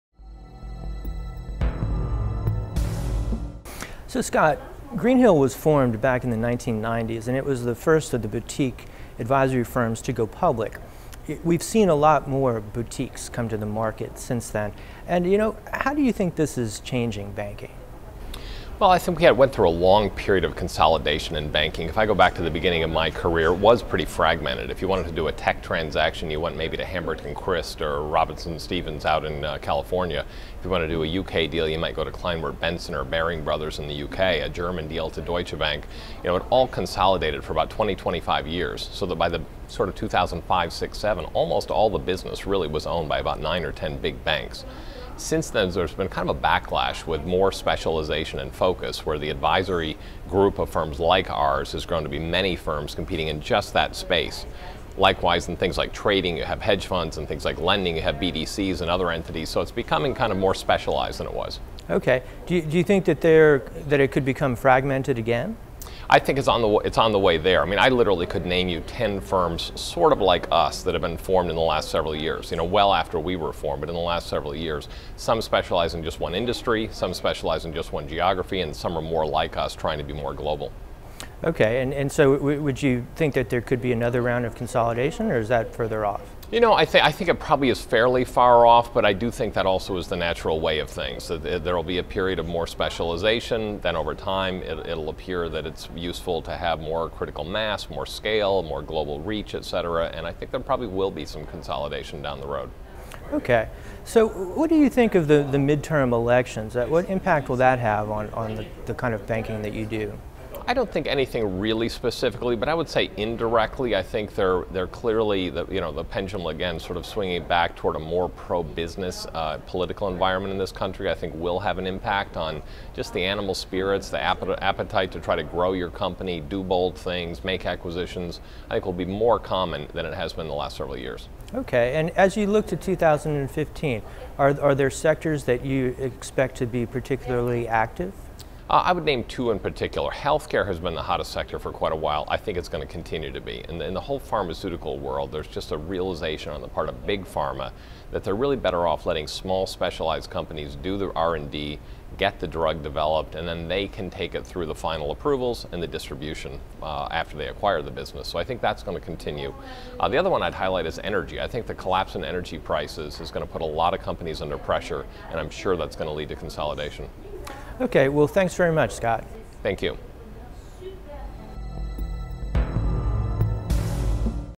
The rise of boutique M&A banks with Greenhill CEO Scott Bok
Greenhill & Co. was the first of the boutique M&A firms to go public. CEO Scott Bok discusses the proliferation of firms that provide à la carte deal advice, without the attached strings of financing.